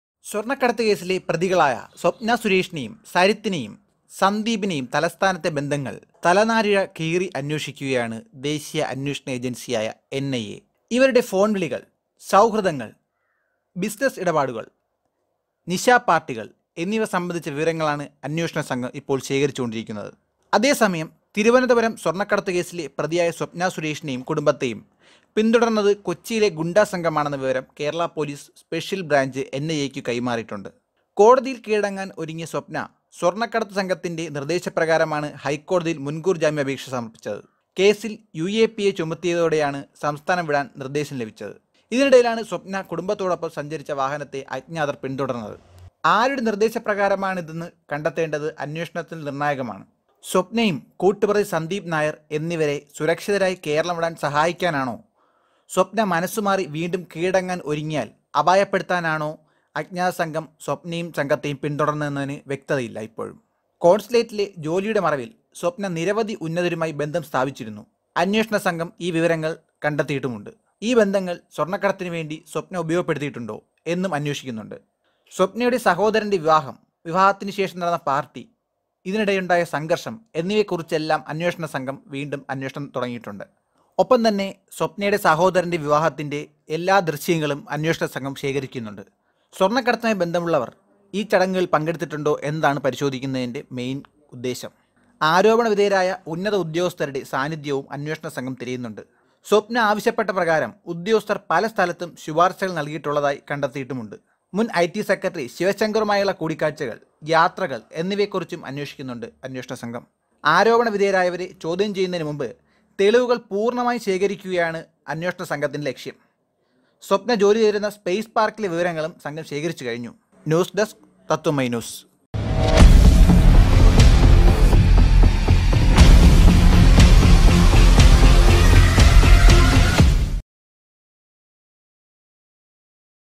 സ്വപ്‌നക്കും ഗുണ്ടാസംഘം? ബന്ധങ്ങള്‍ വിശദമായി അന്വേഷിക്കണം..
സ്വപ്‌നക്കും ഗുണ്ടാസംഘം? ബന്ധങ്ങള്‍ വിശദമായി അന്വേഷിക്കണം.. സ്വപ്ന ആവശ്യപ്പെട്ടപ്രകാരം ഉദ്യോഗസ്ഥര്‍ പല സ്ഥലത്തും ശുപാര്‍ശകള്‍ നടത്തി.ശിവശങ്കറുമായുള്ള കൂടിക്കാഴ്ചകള്‍, യാത്രകള്‍ എന്നിവയെക്കുറിച്ചും അന്വേഷണം.